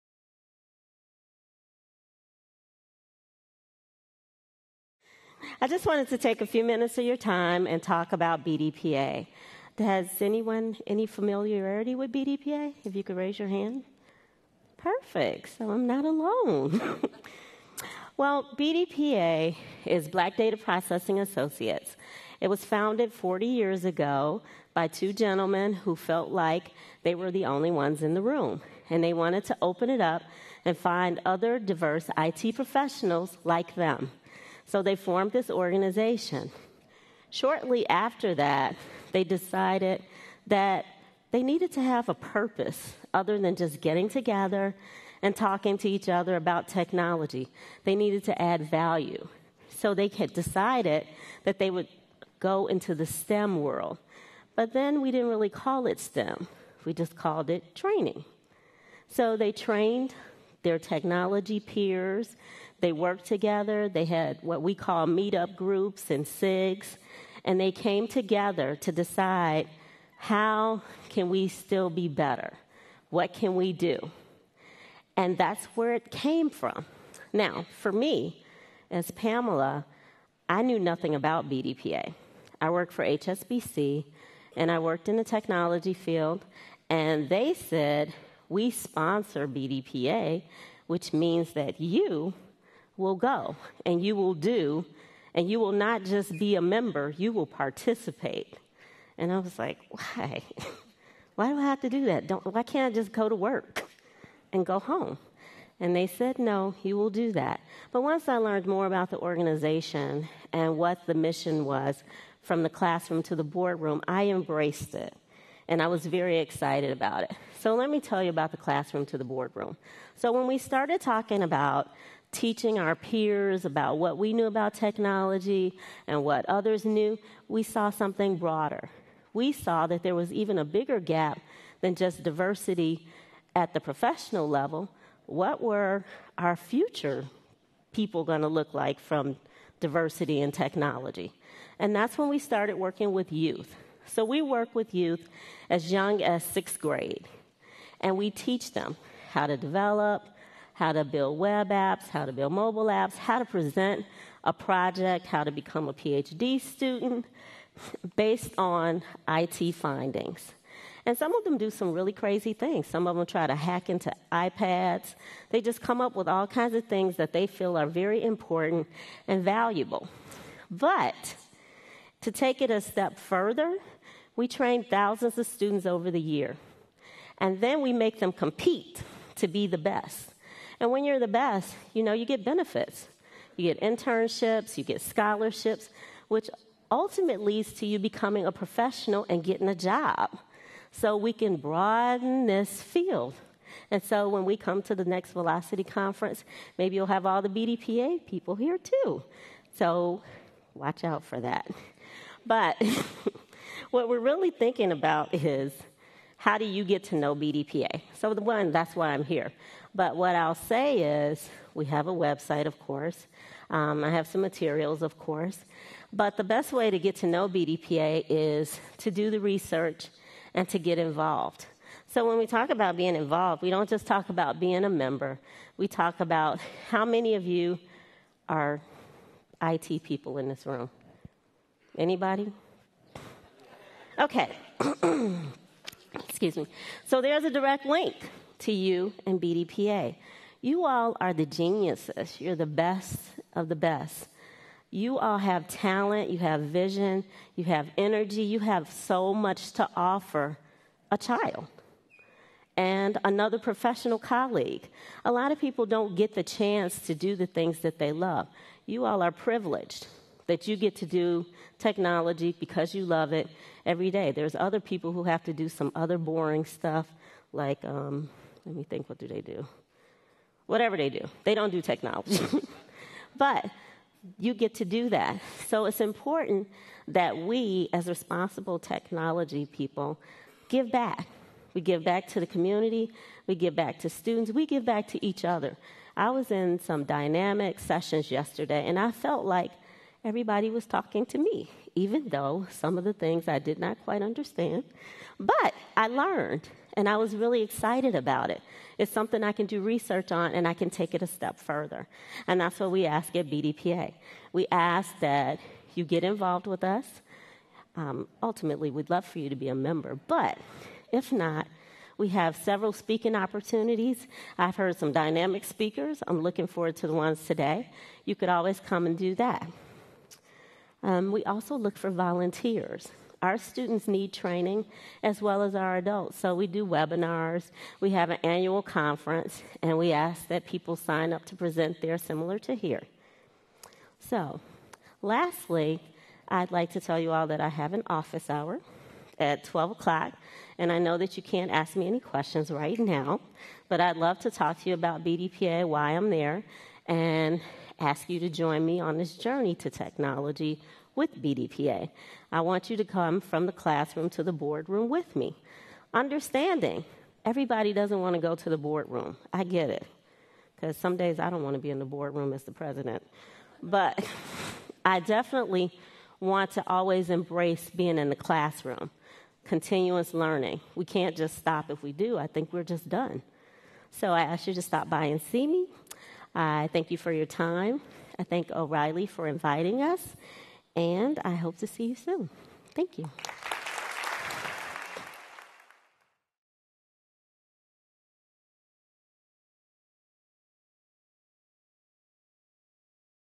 BDPA: From the Classroom to the Boardroom - Pamela Mathews keynote
From the 2015 Velocity Conference in New York, a keynote on the Black Data Processing Associates.